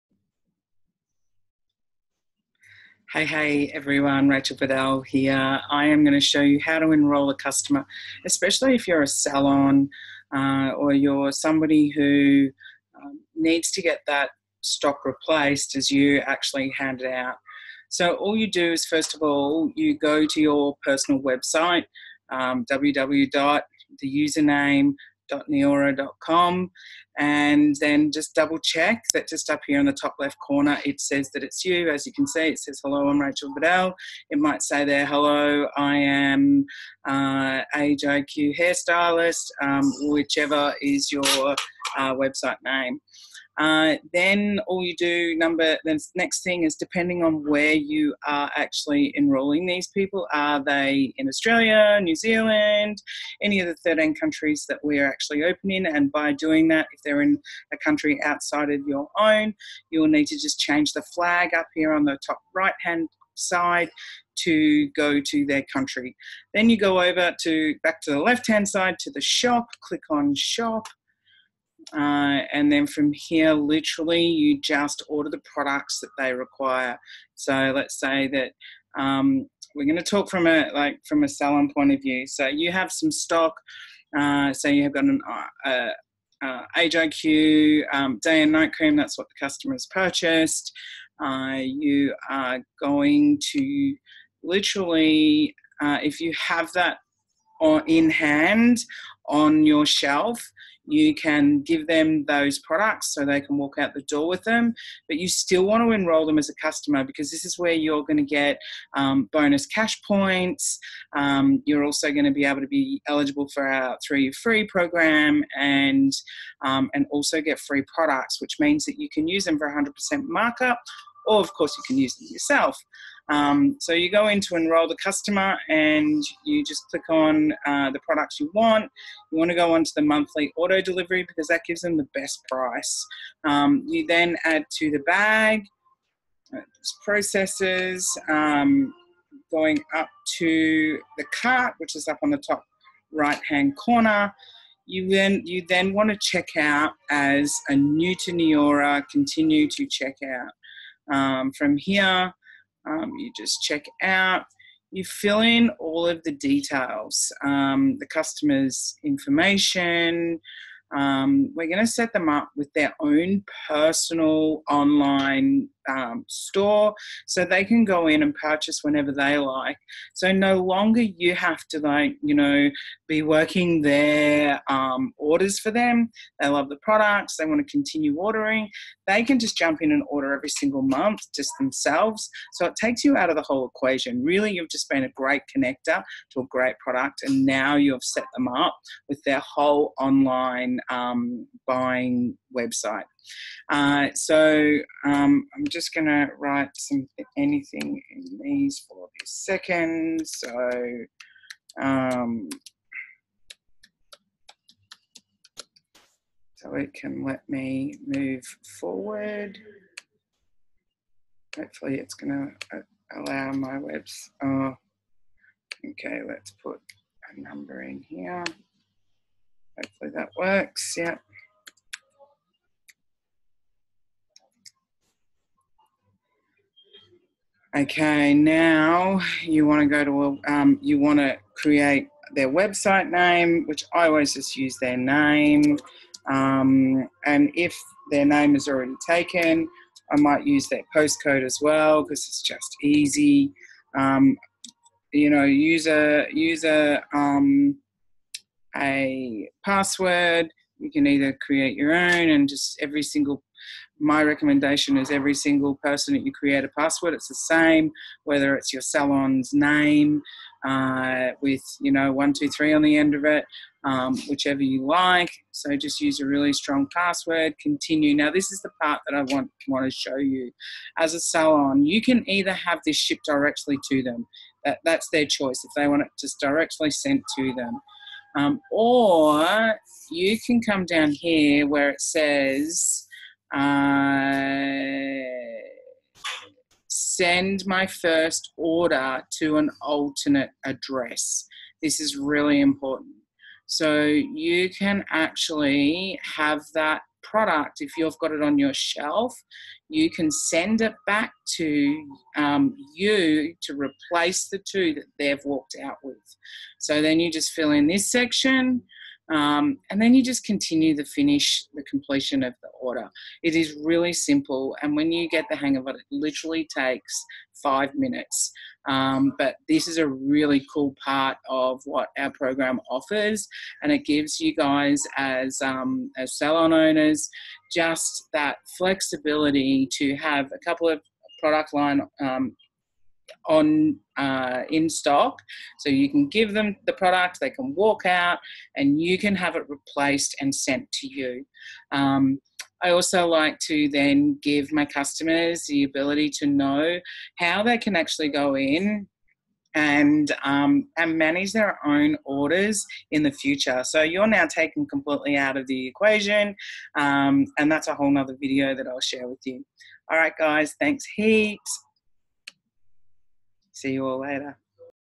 How to Enrol a Customer Salon Owners - Neora
I love this we are the connector of a great product can set our customers up with their own ability to order when they want in the future. The coolest part is their first order we can have in stock and have their first order sent to us to replace. No chasing people its simple and brilliant.

Step 1.
Step 2. Double check its yours/ Change flag to country of customer ordering
Step 3. Go to SHOP and order ADO price is the best price
Step 4. Check out as NEW to Neora follow all prompts to address
Step 5. If you are sending replacement order to your salon check box send to alternate address. then continue to process
Step 6. Give customer their website name
Step 7. Give customer login details to access their shop so they can now order when they like.
Step 8. Enjoy being he connector to an incredible product giving your customer the ability to access when they like.
Step 9. Don't forget to let them know theres a way to get their products for free and enjoy extra cash if they would like to know more.